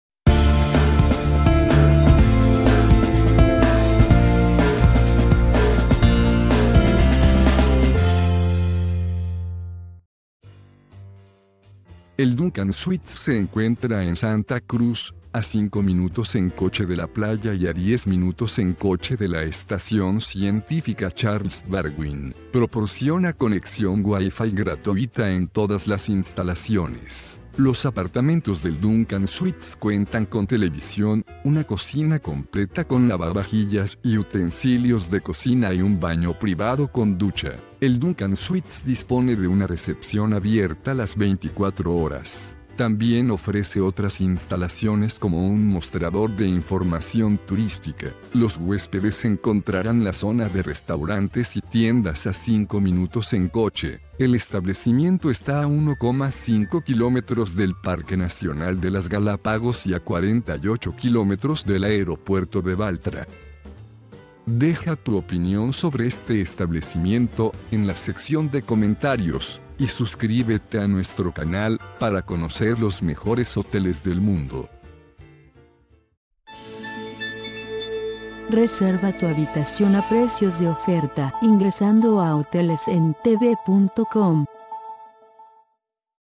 Hostal Duncan en Puerto Ayora
Hostal Duncan goza de recepción 24 horas, servicio de lavandería y habitaciones familiares, además el edifico de Hostal Duncan se halla perfectamente ubicado en una excelente zona de Puerto Ayora.